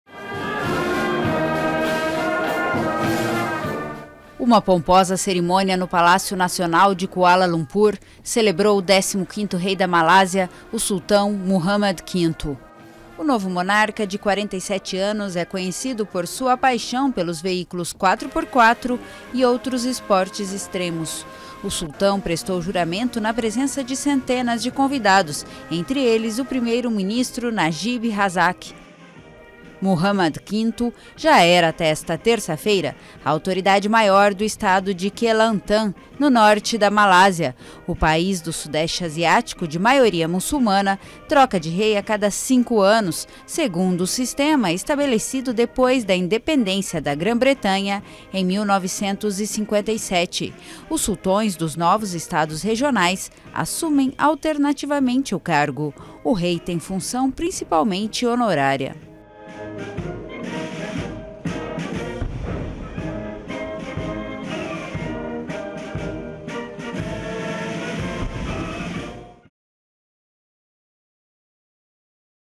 Novo rei da Malásia é coroado
O sultão Muhamad V, assumiu o trono nesta terça-feira em Kuala Lumpur como o 15º rei da Málásia. O monarca de 47 anos era a autoridade maior do Estado de Kelantan, no norte do país.